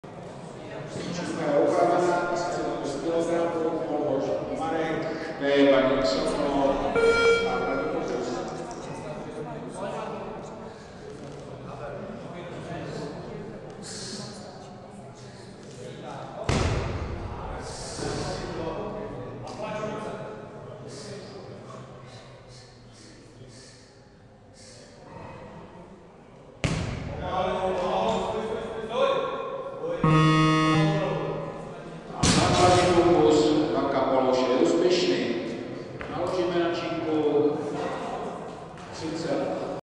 Polhoš Marek (2003), NH1, 35 kg
Finále ligy starších žáků ve vzpírání družstev

Místo: Příbor

Federace: ČSV
Pořadatel: SKV Příbor